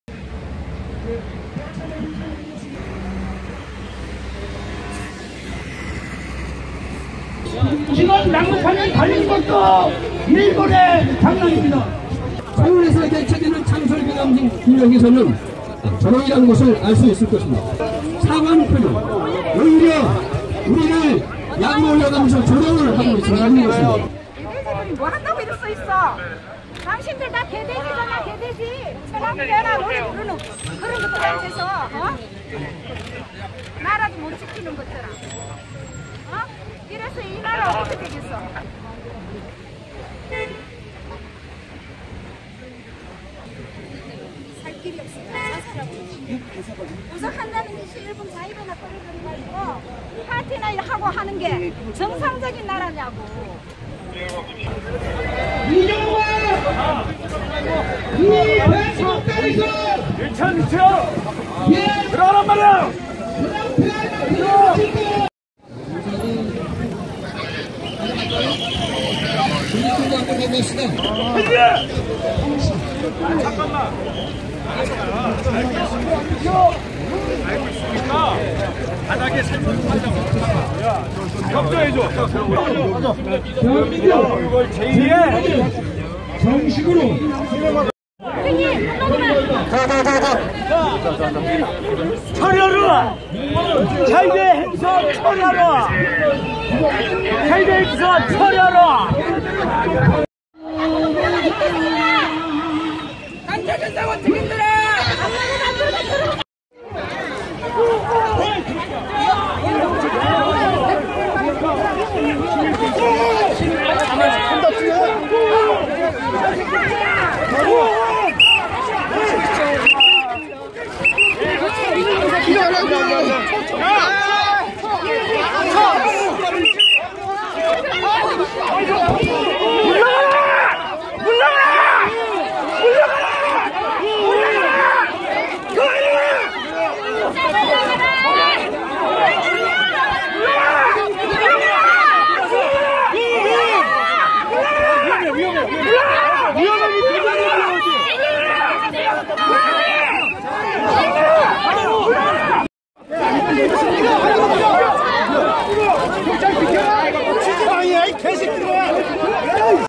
[자위대 행사장] 온 몸 던져 외교부 차량 막는 시민들
자위대 창립 기념 행사가 열리는 서울의 한 호텔.
시민들이 온 몸을 던져 외교부 차량을 막고 있다